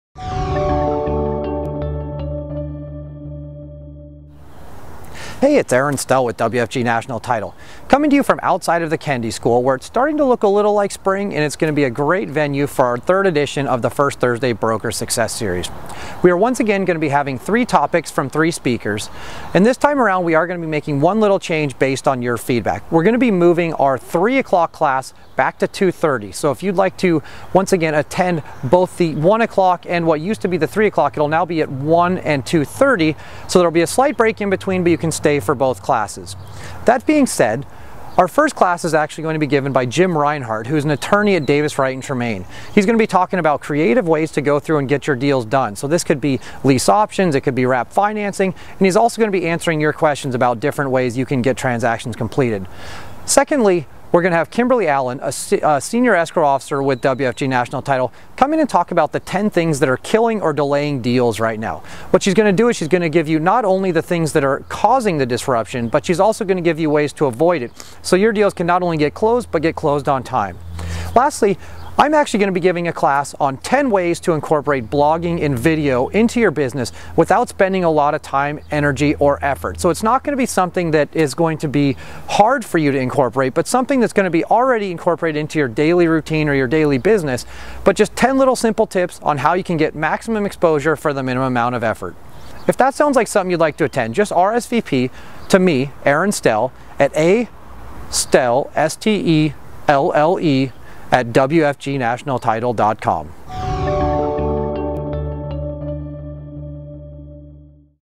WFG First Thursday Broker Success Series Version 3
Date:
First Thursday - May 3rd, 2012

Place:
McMenamins - Kennedy School - Martha Jordan Room
5736 NE 33rd Ave, Portland, OR 97221

Topics:
10am Non-Traditional Ways To Get Your Deal Through - APPROVED HOUR
James C. Reinhart - Davis Wright Tremaine LLP

1pm 10 Things That are Killing or Delaying Transactions and How to Avoid Them - APPROVED HOUR
Kimberly Allen - WFG Senior Escrow Officer

2:30pm 10 Simple Ways to Use a Blog or Video to Get Business
Aaron Stelle - WFG Lloyd Office Sales Representative